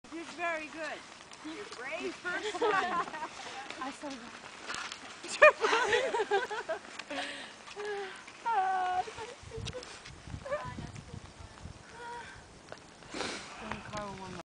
Cross Country in Canada
Yasmin's first time Cross Country Skiing